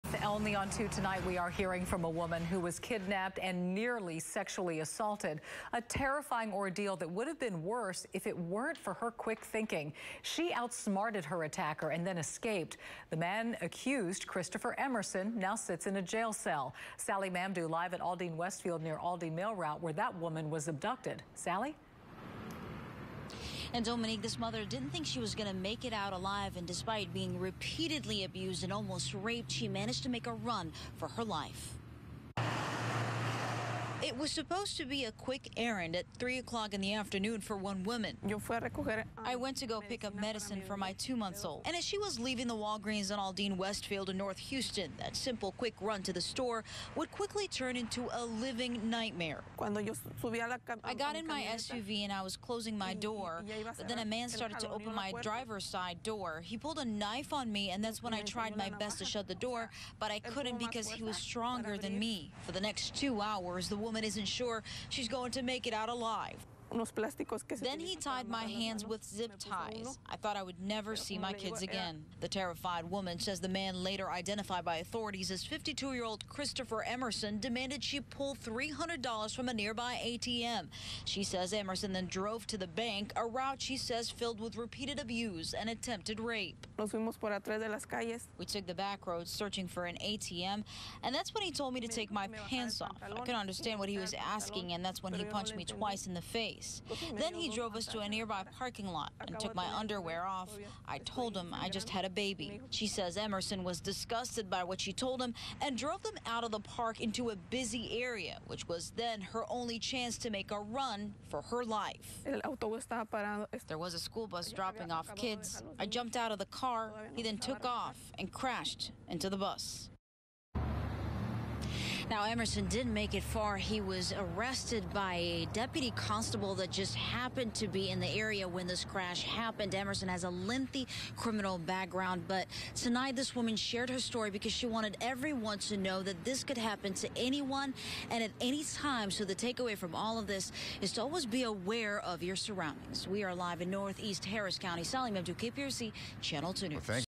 Woman kidnapped, nearly assaulted
A woman who was kidnapped talks about how she escaped her abductor. The accused kidnapper appeared in court on Thursday.